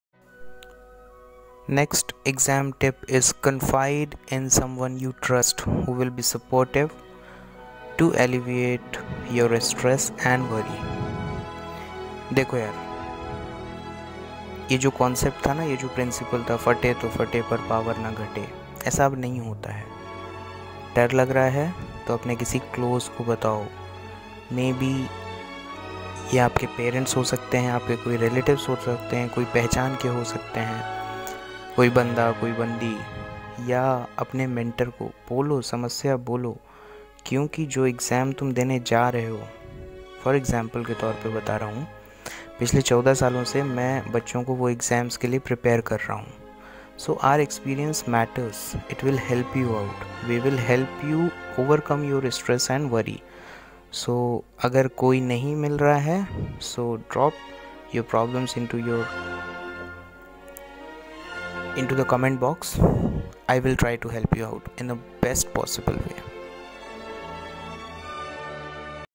Exam Tip No. 4 | Confide In Someone You Trust | फटे तो फटे पावर ना घाटे | Student Talks
इससे आपका दिल भी हल्का होगा और हो सकता है की सम्भावना ये भी है की आपको आपकी समस्या का हल भी मिल जाये।